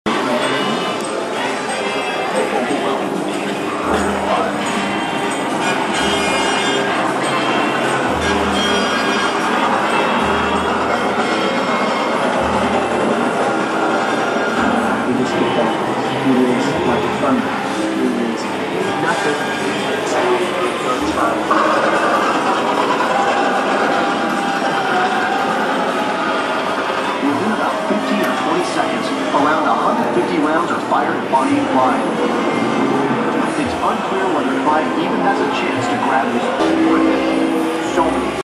Awesome Bonnie and Clyde Car
Awesome bullet hole ridden Bonnie and Clyde Car.